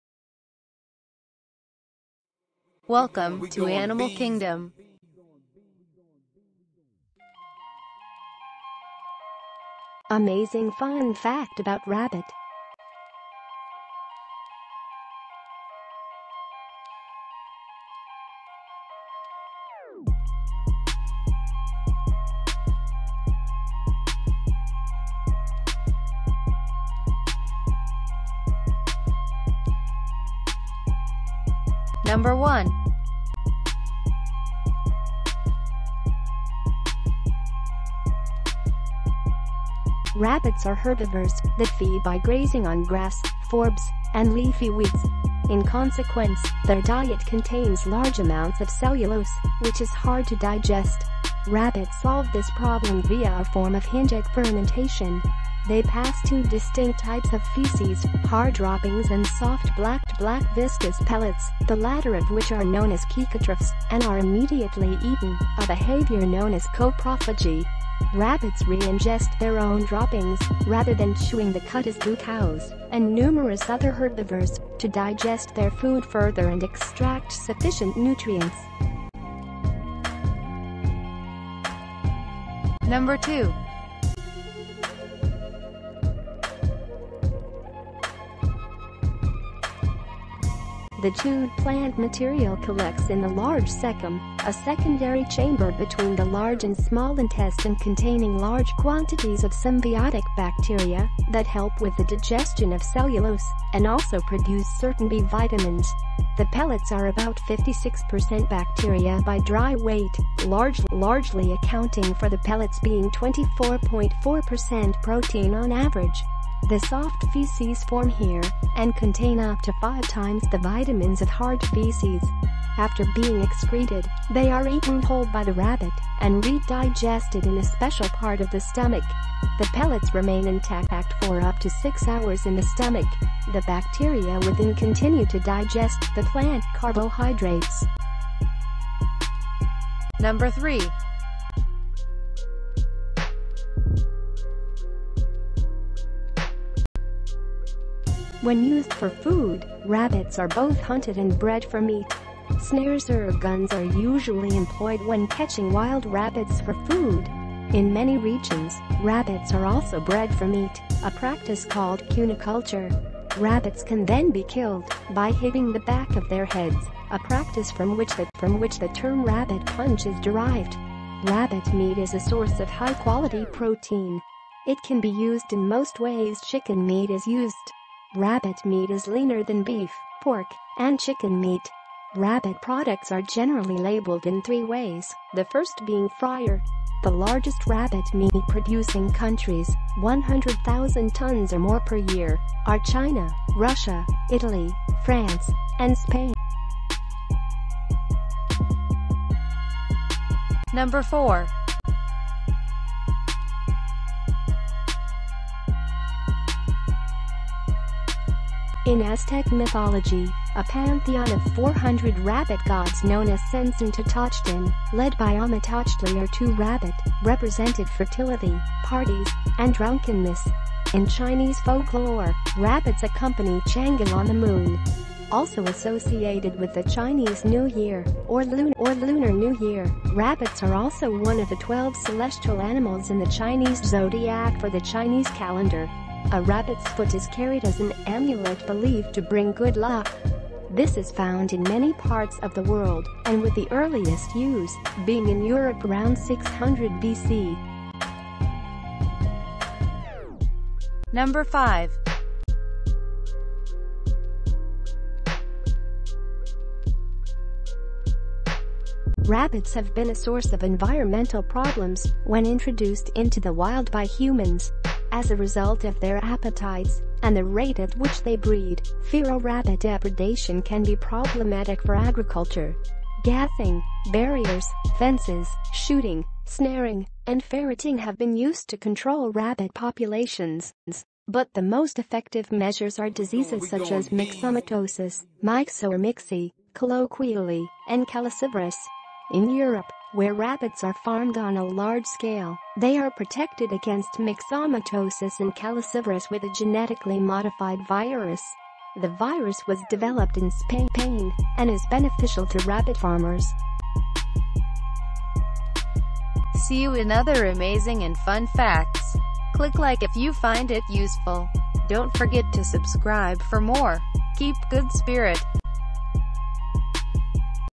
Animal Kingdom - Amazing Fun Fact about Rabbit – Part 2
Animal Kingdom - Animal Facts
Amazing Fun Fact about Rabbit – Part 2

Rabbits are herbivores that feed by grazing on grass, forbs, and leafy weeds. In consequence, their diet contains large amounts of cellulose, which is hard to digest. Rabbits solve this problem via a form of hindgut fermentation. They pass two distinct types of feces: hard droppings and soft black viscous pellets, the latter of which are known as caecotrophs and are immediately eaten (a behaviour known as coprophagy).

Don't forget click like if you find it useful...